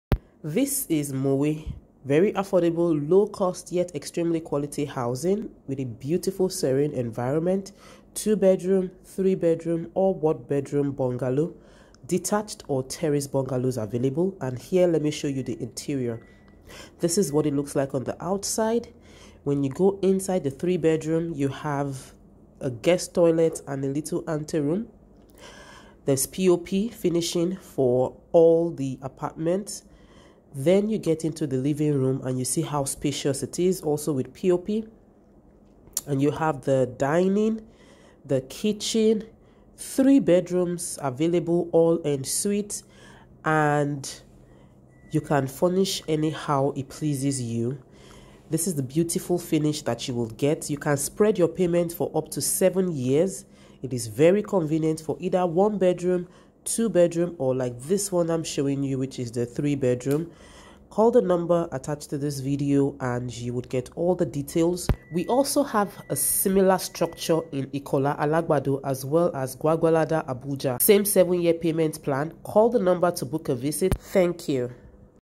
Quality Housing in Mowe, Ogun state Nigeria with Flexible payment plan
The Building is locate right in Mowe, Ogun state Nigeria. The payment plan is within 7 years. The environment is serene an beautiful and secured.